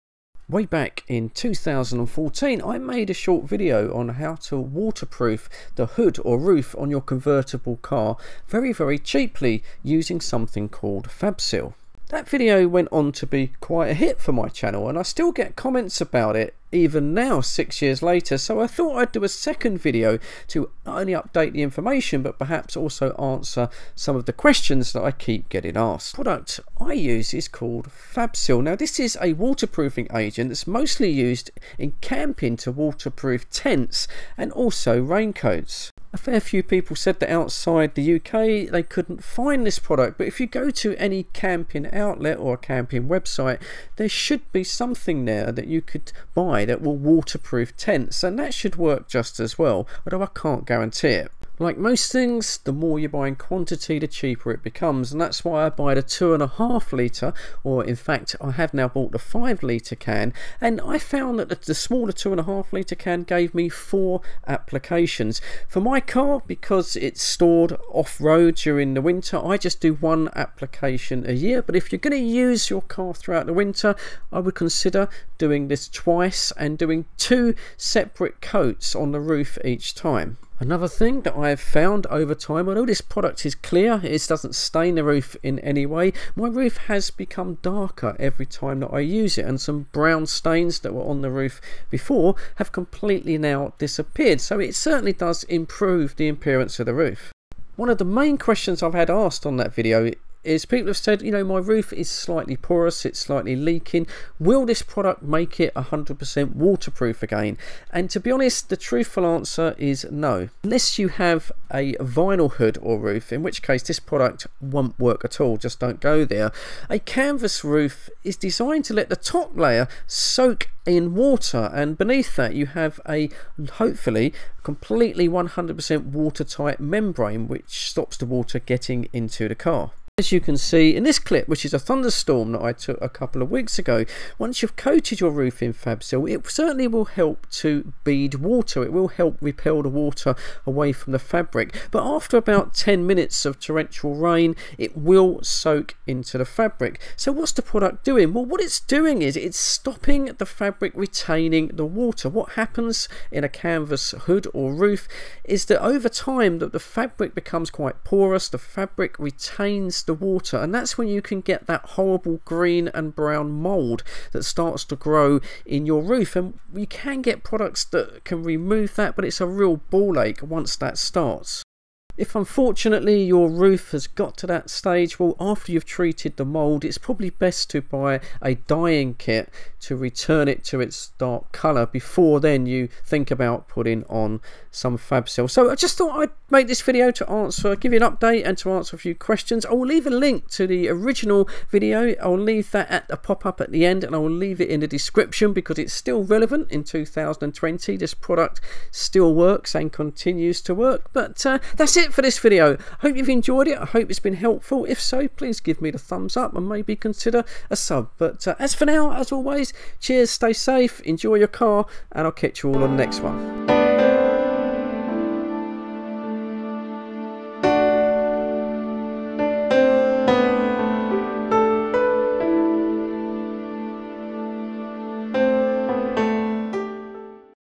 Waterproofing a convertible cabriolet roof cheap. Update 2020.
An update / questions answered on how to cheaply Waterproof a convertible cabriolet roof. Orig guide video